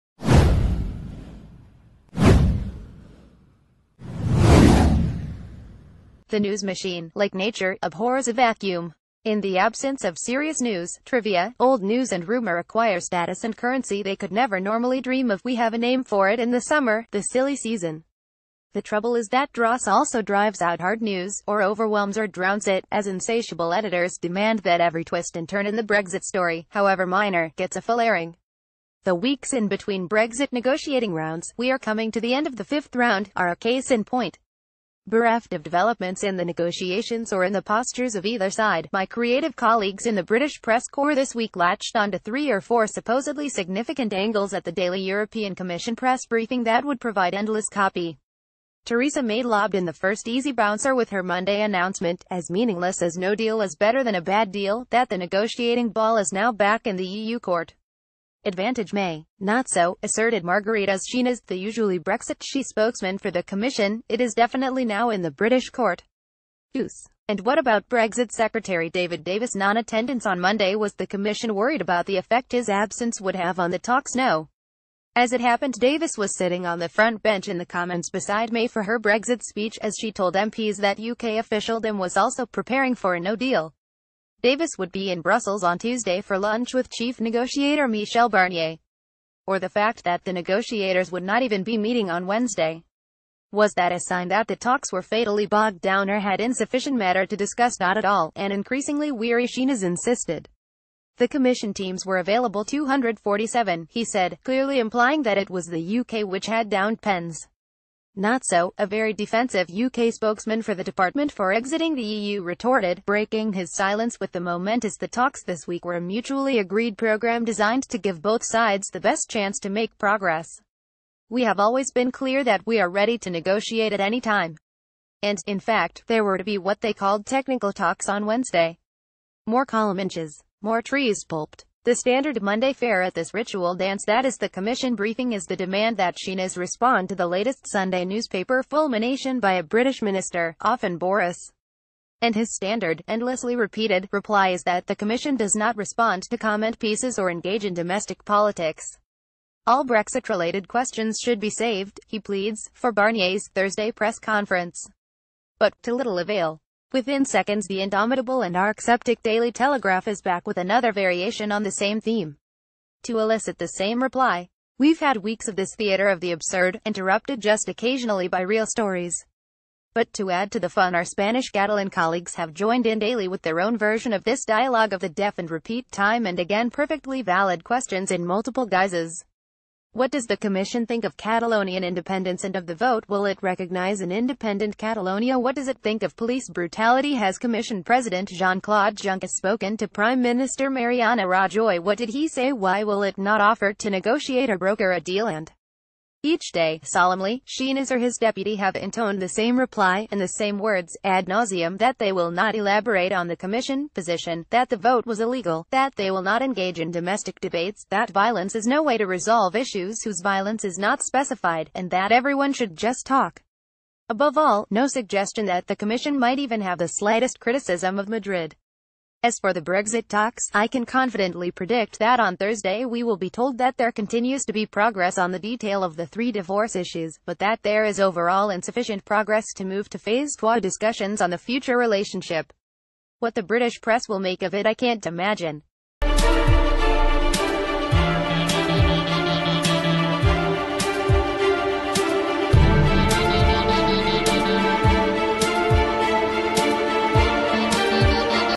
Breaking News | Theatre of the absurd in ritual dance between eu and british press
Theatre of the absurd in ritual dance between eu and british press

The news machine, like nature, abhors a vacuum. In the absence of serious news, trivia, old news and...